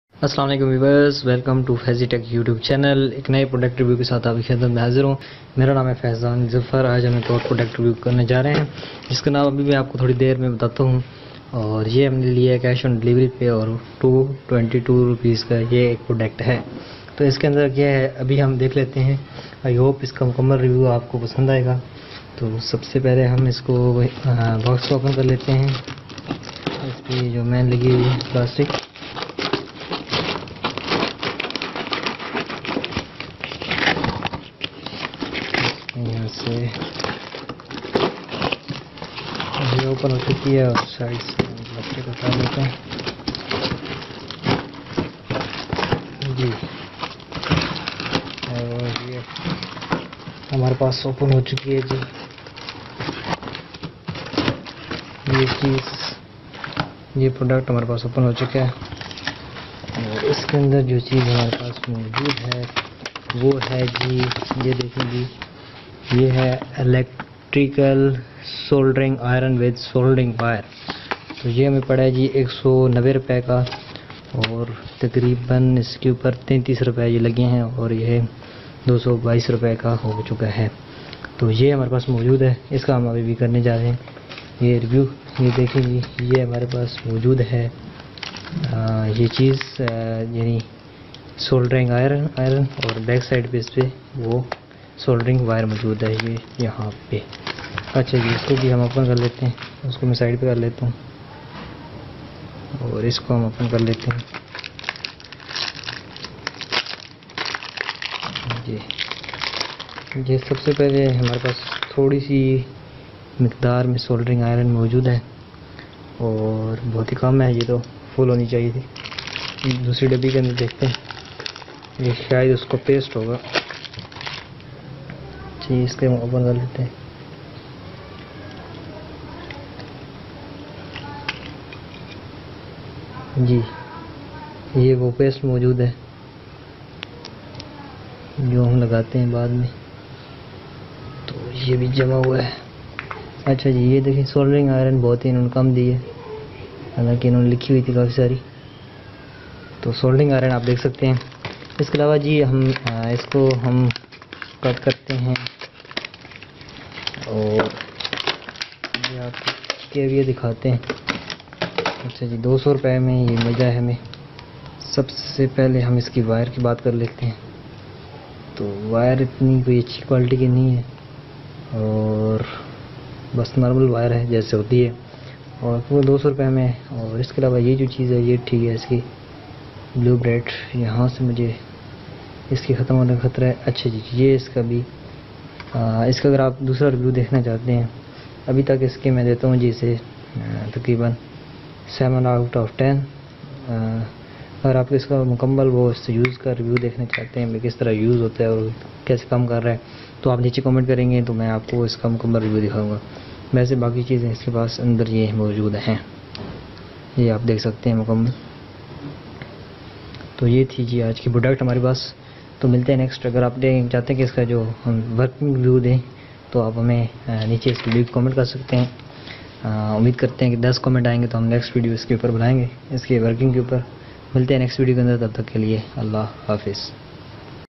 Electrical Soldering Iron With Soldering Wire - Daraz.pk Product - Honest Product Review
Assalam-o-Alaikum Kasa Hain dosto app is video ka andar
Electrical SolderingIron With SolderingWire - Daraz.pk Product - Honest Product Review
Mukamal Information ka lia  is video ko akhair taak daikta rahain sukira.

Video Courses
Inpage 2009 Complete Course class 1 of 20

If you like Fazie Tech and Add Your Detail on This Map